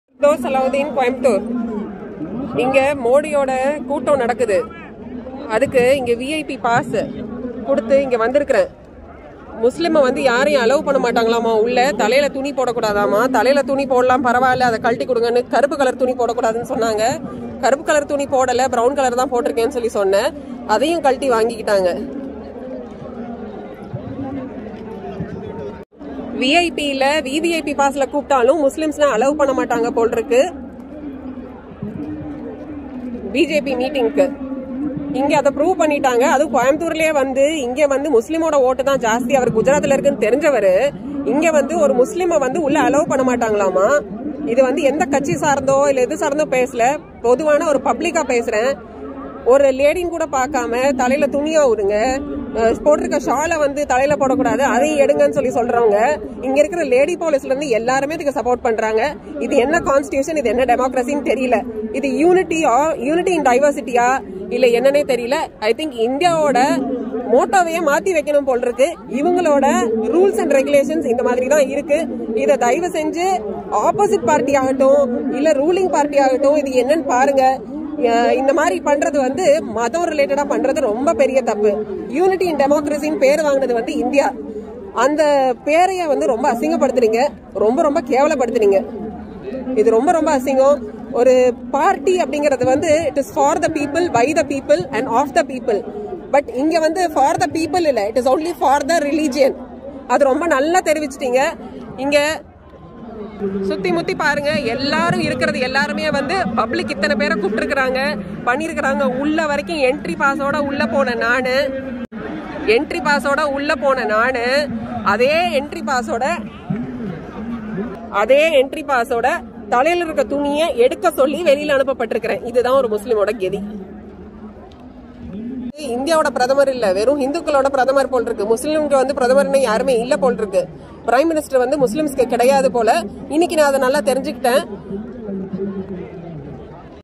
பா.ஜ.க பொதுக்கூட்டத்தில் இஸ்லாமிய பெண்ணுக்கு அனுமதி மறுப்பு
கோவை பா.ஜ.க பொதுக்கூட்டத்தில் இஸ்லாமிய பெண்னை அனுமதிக்க காவல் துறை மறுப்பு

 என தகவல்.. தலையில் மத அடையாளத்துடன் கூட்டத்திற்கு செல்ல அனுமதி மறுக்கப்பட்டதாக இஸ்லாமிய பெண் தகவல்